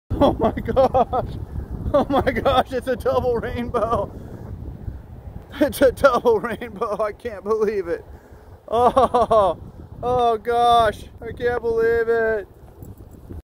Otherwise normal man completely loses it over majestic double rainbow 🌈! Warning: Disturbing content
Double rainbow. Man shamefully overreacts. Disturbing. Remove children before watching.